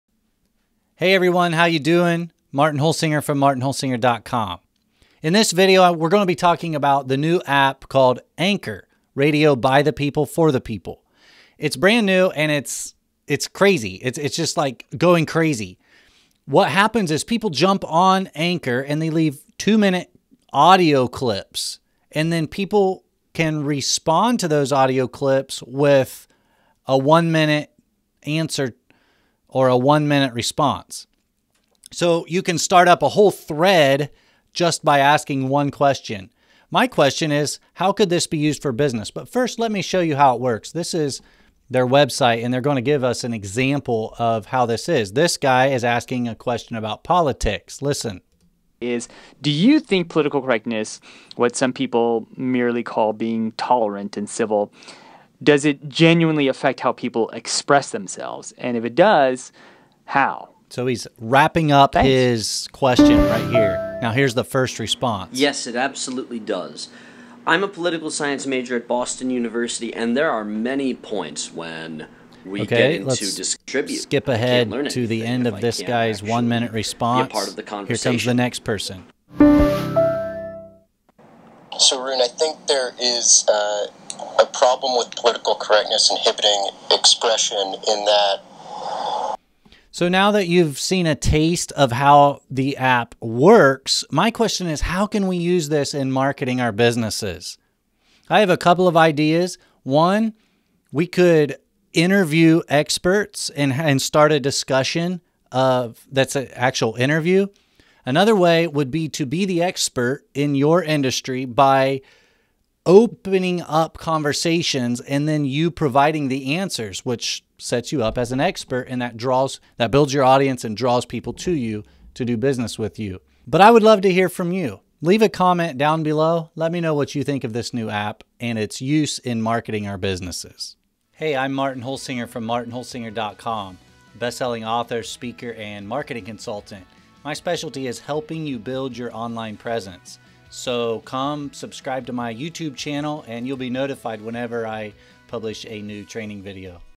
Anchor App Marketing for Business
I'm so intrigued with Anchor app right now. It's taking off like a firestorm. Here's a couple of ideas of how you can use the anchor app for marketing.

I want to hear your ideas in the comment section below.

Are you using Anchor app?
Are you using it to market your business?

SCHEDULE

* Fri -  Marketing Tips: